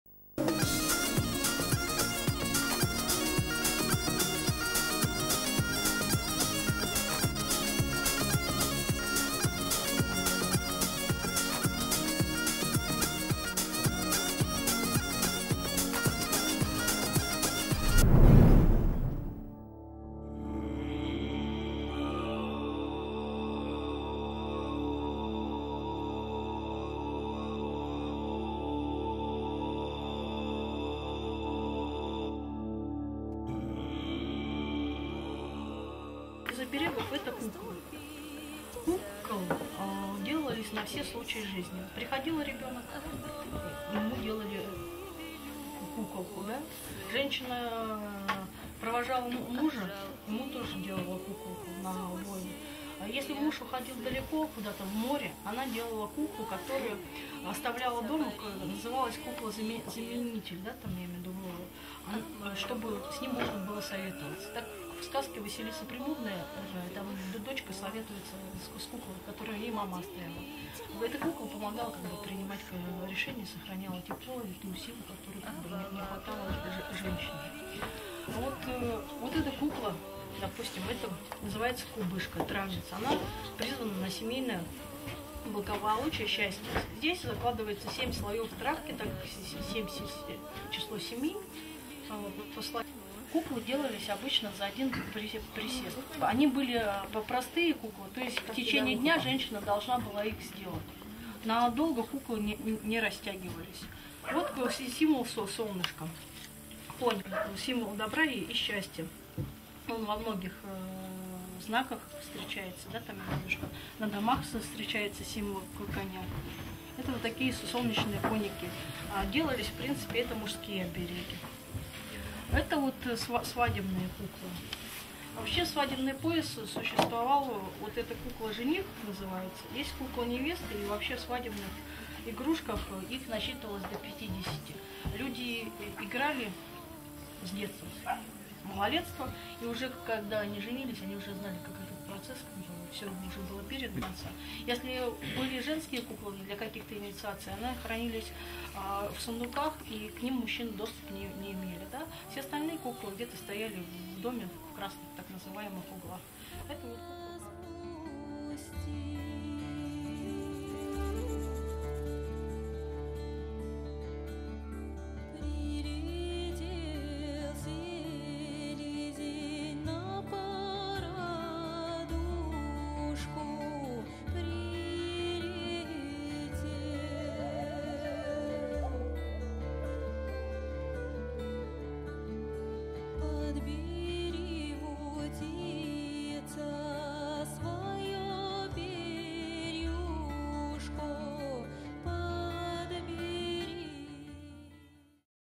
Russian rag-doll.
Russian rag-doll. What do we know about it? What meaning it has? What can it give to its owner? One cannot imagine what a huge unexplored world of our ancestors can be opened while making of this simple at first sight doll. There is a good but unfortunately forgotten proverb in Russia: One who does not play in dolls, does not know happiness.

For ages rag-doll was not just a toy, it was a symbol of clan continuation, pledge of family happiness, health and wealth.  While education of kids a lot of attention was paid to their playing with rag dolls. While paying kid became acquainted with himself and the surrounding world; was taught to keep house; and made his mind for understanding of a word FAMILY.

Rag doll is a helper; a guard; a symbol. And in our unstable time this revived symbol can change a lot in everyday burden of life. Beautify your world, bring some light and happiness to it; and the surrounding world will smile back to you.